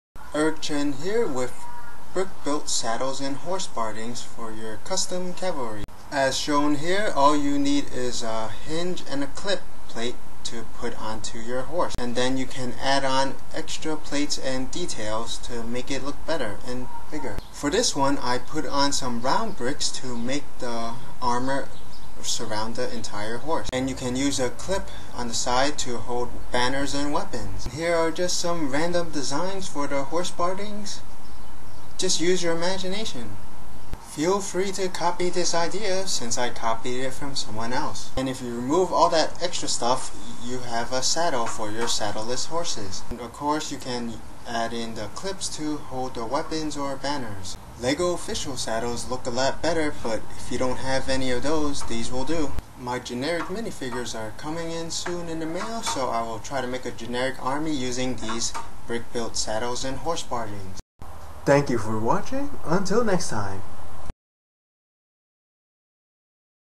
Brickbuilt Horse Saddle and Barding Tutorial
I made my own variations of  these saddles and horse armor/bardings based on designs by boses from classic castle and Rev. Sylvanus  from Brik Wars.

Generic army video sometime soon!